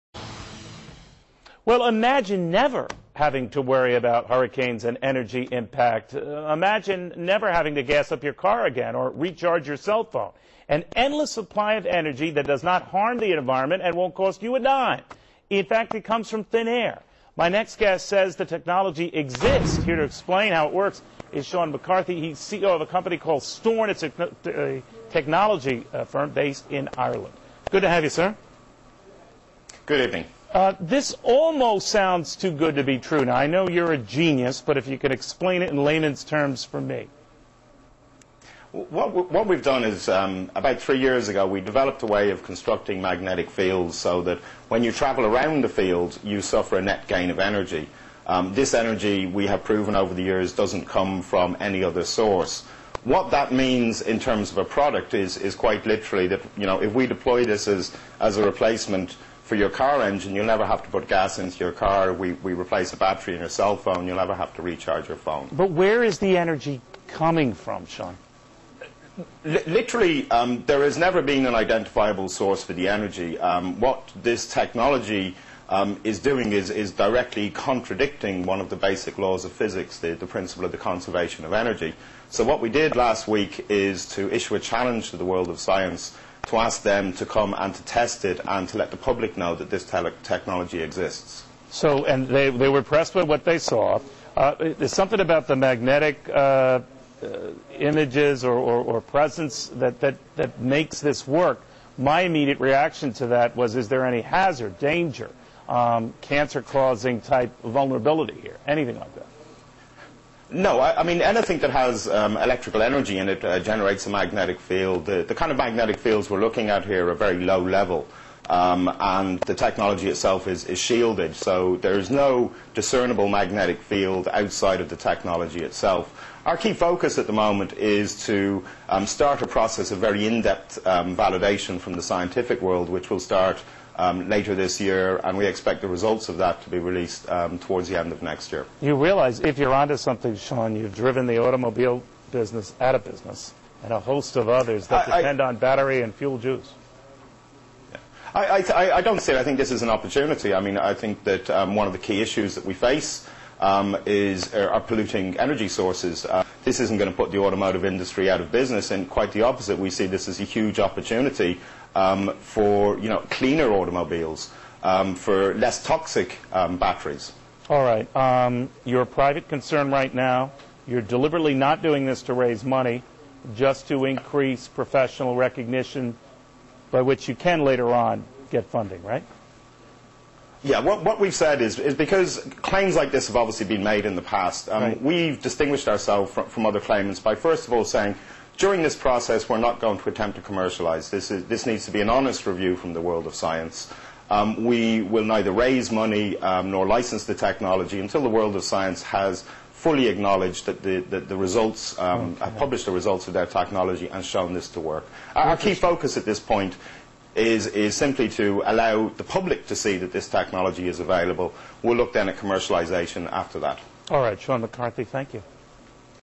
Steorn Power on Fox News Interview
Steorn will launch a revolutionary free, clean, energy technology.  for more up2date news